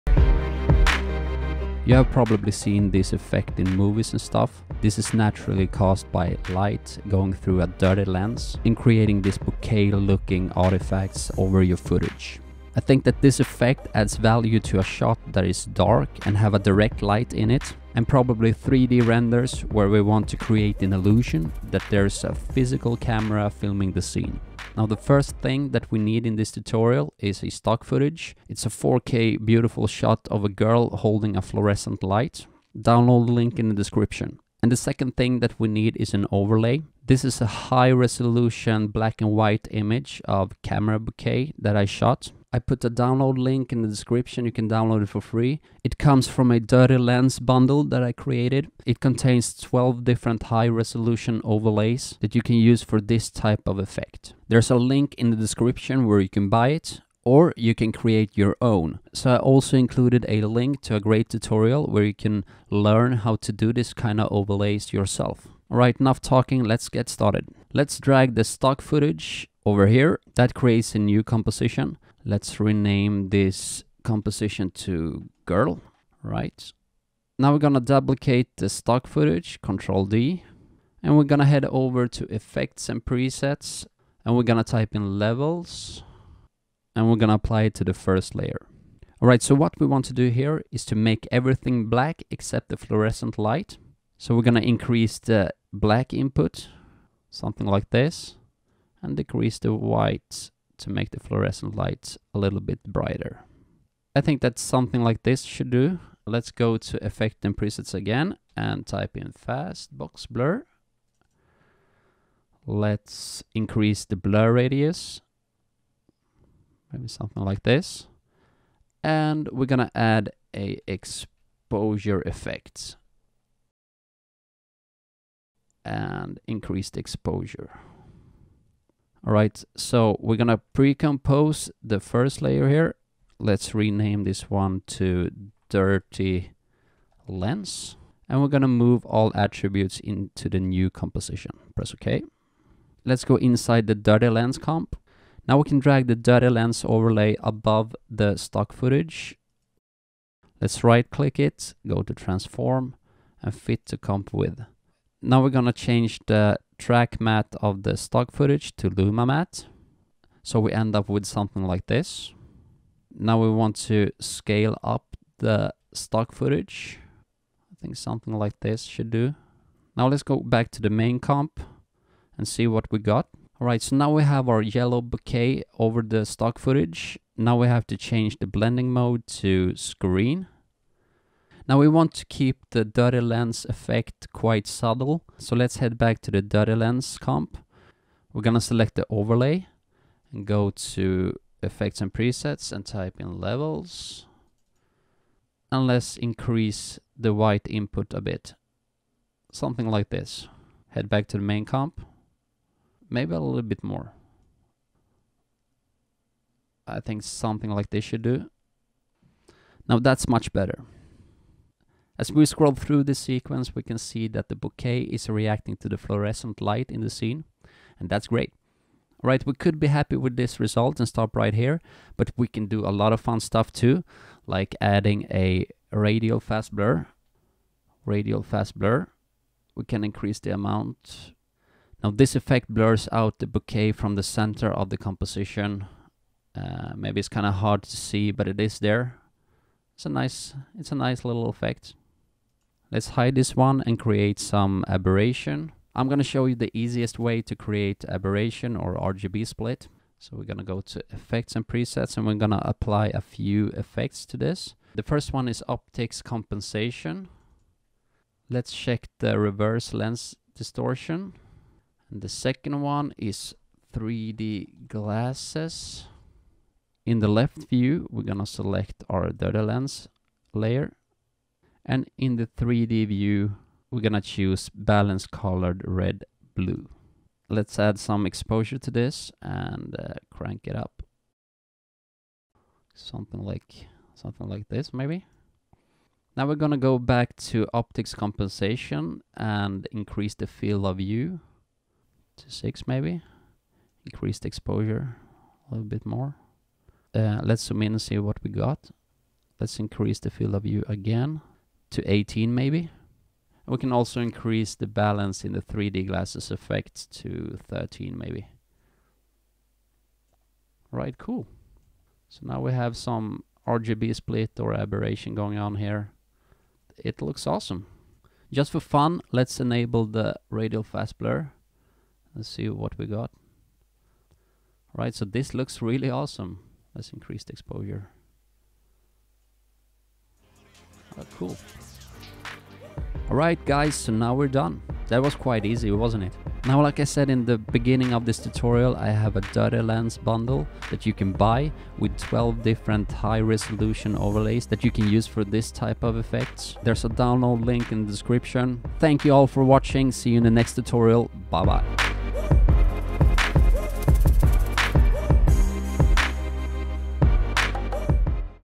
Dirty Lens Effect - After Effects Tutorial
Create a dirty lens effect and add something extra to your videos. This tutorial shows you how easy it is to create such effect in After Effects.

Dirty Lens Bundle contains 12 high resolution luma matte overlays. The image size of 5184 x 3456 pixels makes the overlays perfect to use in 4K videos and photos.